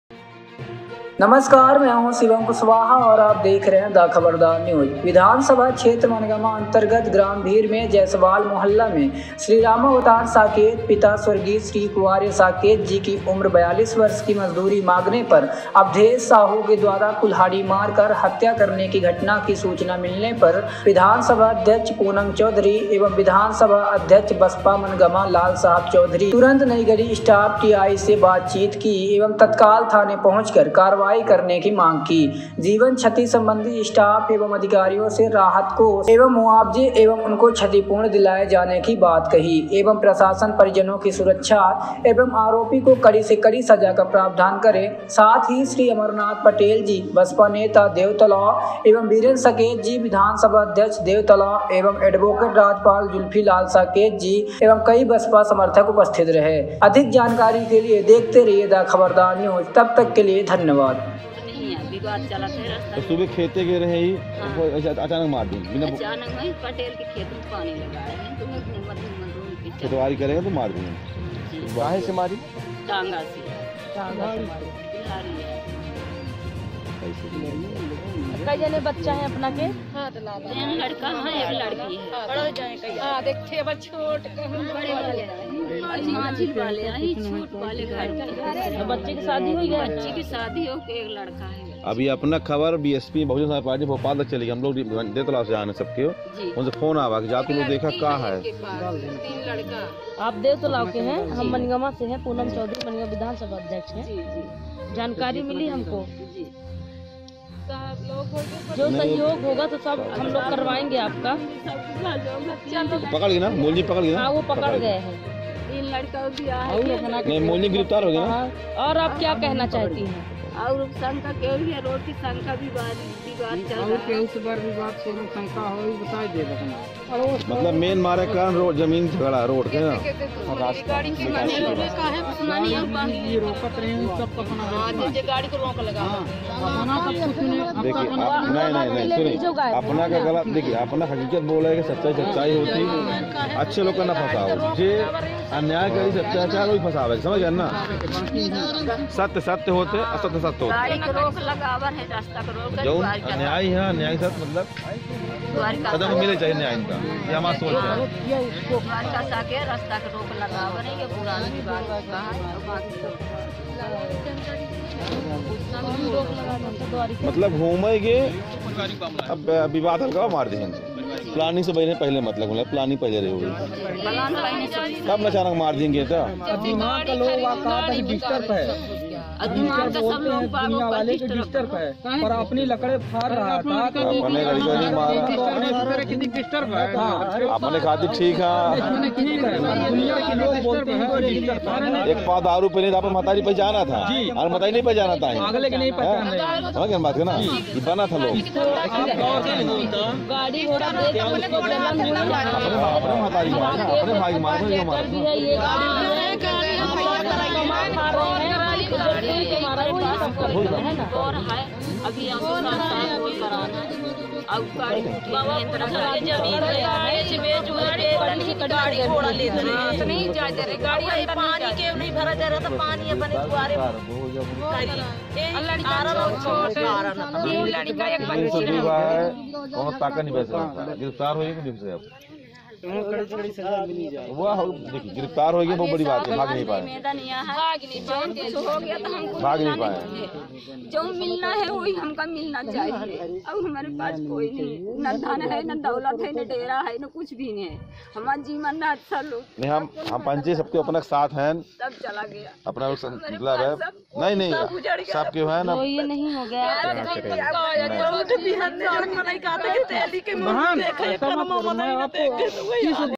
रीवा मनगवां ;मजदूरी मांगने पर मजदूर की कुल्हाड़ी मारकर की हत्या
विधानसभा क्षेत्र मनगवां  अंतर्गत ग्राम भीर  जयसवाल मोहल्ला में  श्री राम अवतार साकेत (मृतक) पिता  स्व. श्री कुआरे  साकेत जी उम्र 42 वर्ष  की  मजदूरी मांगने पर  अवधेश साहू  के द्वारा कुल्हाड़ी मारकर  हत्या करने  की घटना की सूचना मिलने पर विधानसभा अध्यक्ष पूनम चौधरी विधानसभा अध्यक्ष बसपा मंनगवा 73 (लाल साहब चौधरी) तुरंत नईगढ़ी स्टाफ टीआई से बातचीत की एवं तत्काल थाने पहुंचकर करवाई की मांग की ।  जीवन क्षति संबंधी स्टाफ एवं अधिकारियों से राहत कोष एवं मुआवजे एवं उनको क्षतिपूर्ति दिलाएं जाने की बात की। एवं प्रशासन परिजनों की सुरक्षा एवं आरोपी को कड़ी से कड़ी सजा का प्रावधान करें ।  साथ ही  श्री अमरनाथ पटेल जी बसपा नेता  देवतालाव एवं वीरेंद्र साकेत जी विधानसभा अध्यक्ष देवतालाब एव एडवोकेट राजपाल जुल्फी लाल साकेत जी एवं कई  बसपा समर्थक उपस्थित रहे।